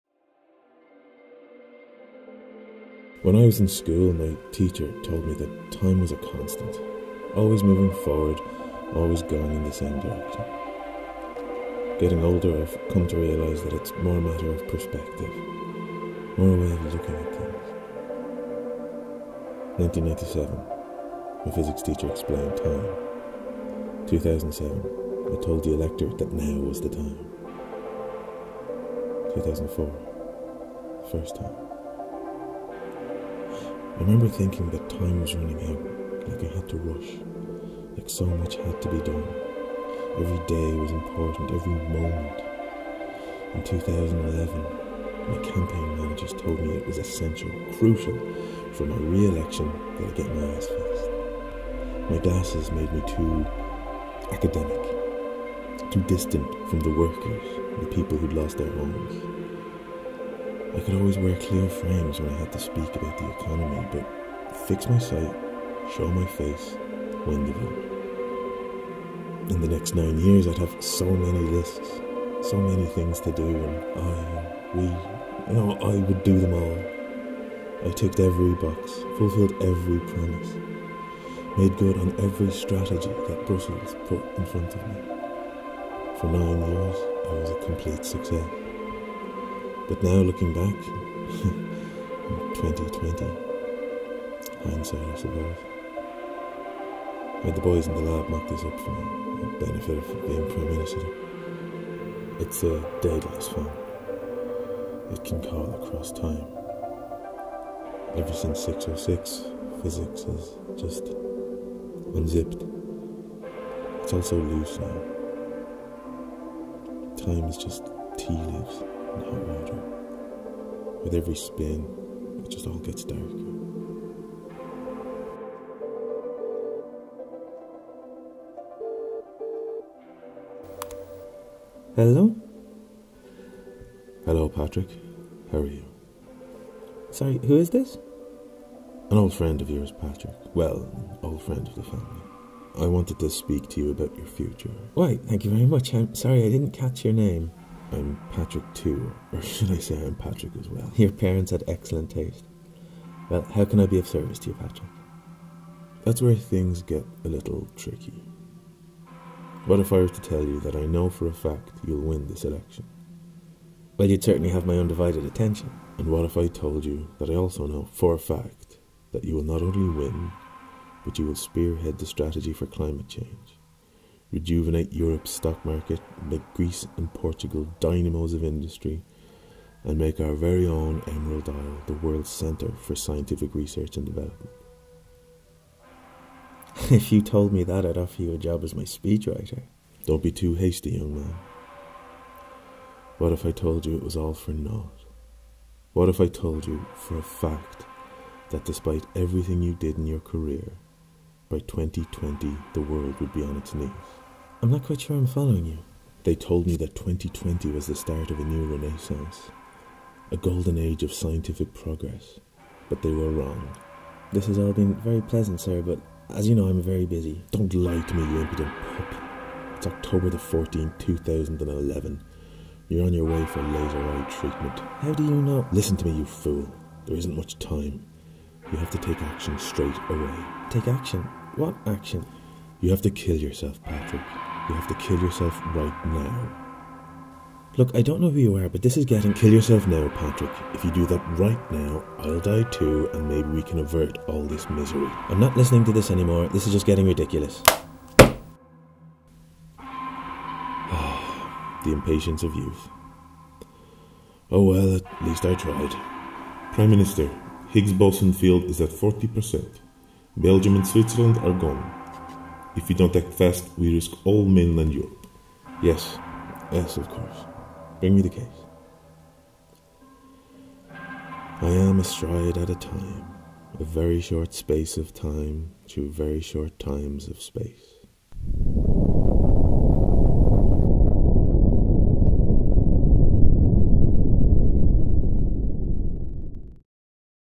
The Strandentwining Cord
The title is take from a passage of James Joyce's Ulysses where Stephen Dedalus imagines using a phone to contact the Aleph; the origin. This short is about origins and time about talking and twins... of a sort. It is about strategy and sight, both fore and hind and also about what happens to the best laid plans.

This film was made for a competition... but I didn't read the small print... so now I have a weird film about Europe that coulda been a contender... but isn't.

Comments disabled but feel free to PM me